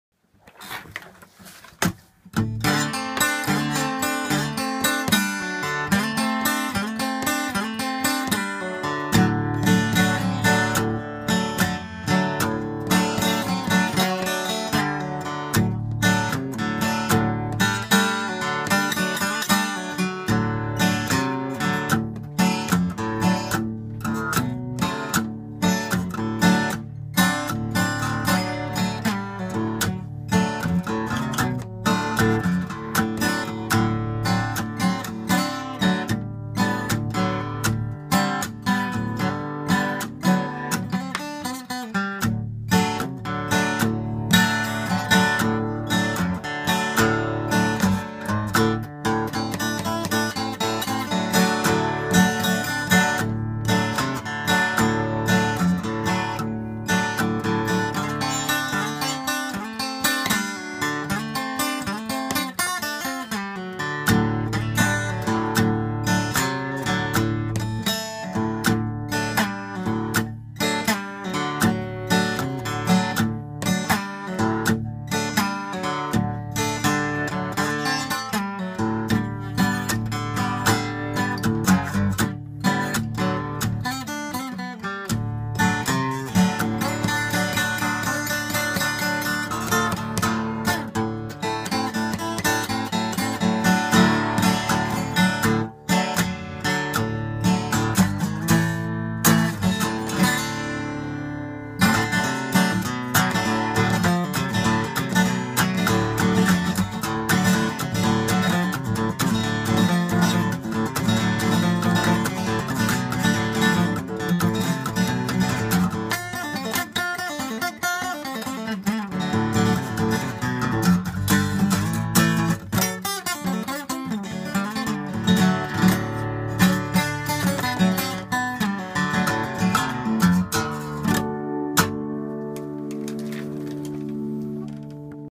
Waking Up My Sigma DR-35
Dusting off and getting reacquainted with my old Sigma DR-35. She's been with me a long time, so it's nice to get back at at. Rough stuff in open D.